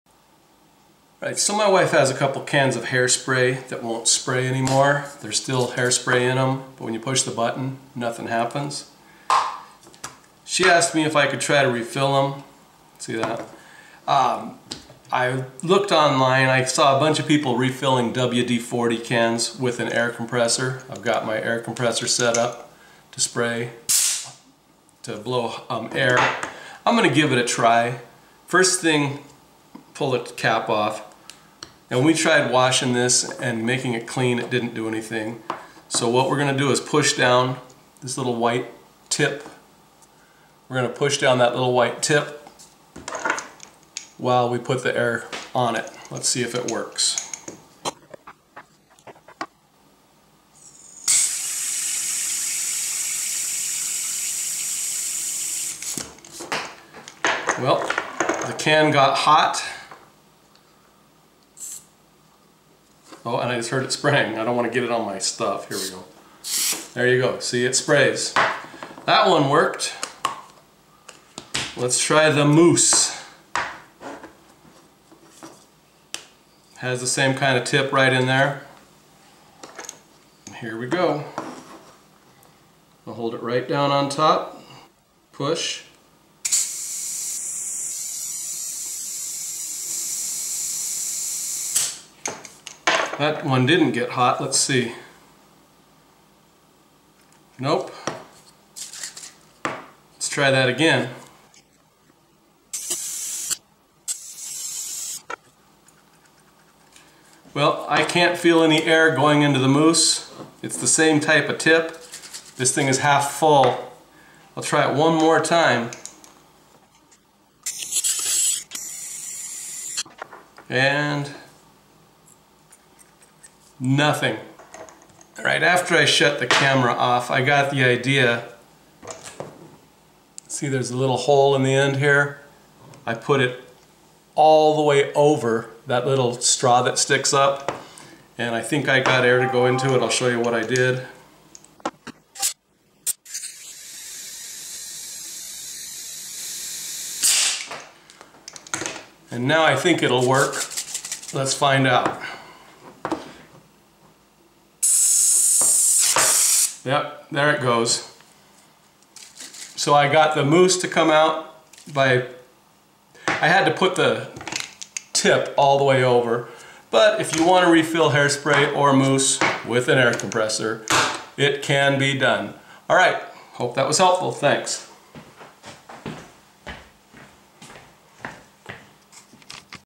How to Recharge Hairspray and Mousse Cans when Out of Air
My wife's hairspray and mousse cans were still half full, but out of air.  Here's how to put more air in the cans so they will spray.  My wife thought it was funny that I didn't know you're supposed to hold the mousse can upside down to spray it.  I hope you find this useful.